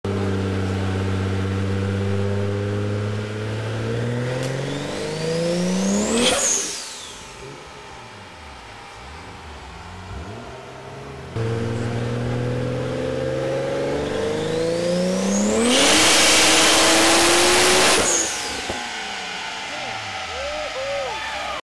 toyota supra turbo 800 hp dyno testing
this is a toyota supra fully charged 800hp at bryces dyno...listen to that sound...